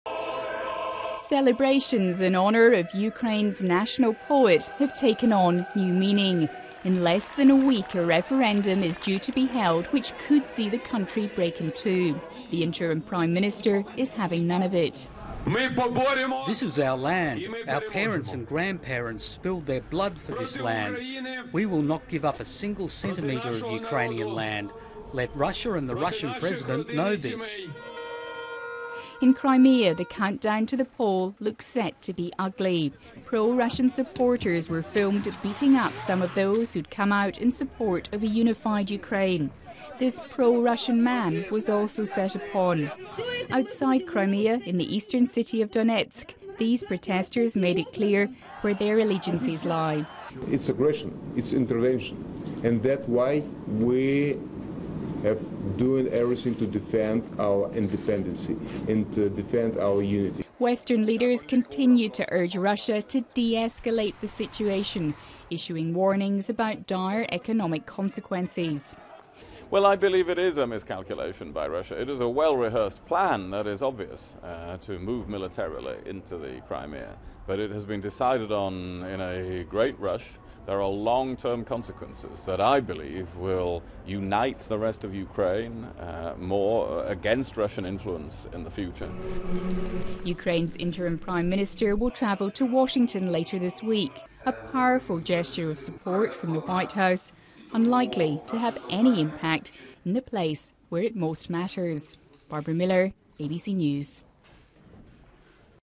Ukraine: Pro Russian and pro Ukranianian demonstrators clash in Sevastopol
10/03/14 The mood in Crimea is tense less than a week out from a referendum on whether the region should secede from Ukraine and become part of Russia.